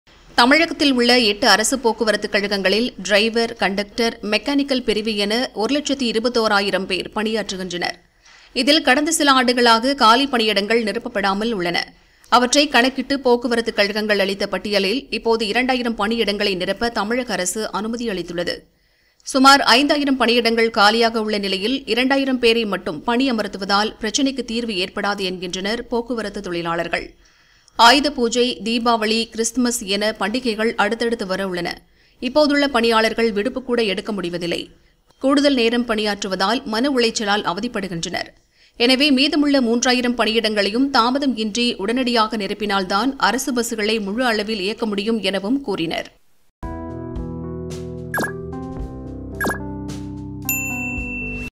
2000 பணியிடங்களை மட்டும் நிரப்புவதால் போக்குவரத்து தொழிலாளர்கள் அதிருப்தி | TNSTC | Recruitment
2000 பணியிடங்களை மட்டும் நிரப்புவதால் போக்குவரத்து தொழிலாளர்கள் அதிருப்தி | TNSTC | Recruitment | driver | conductor | jobs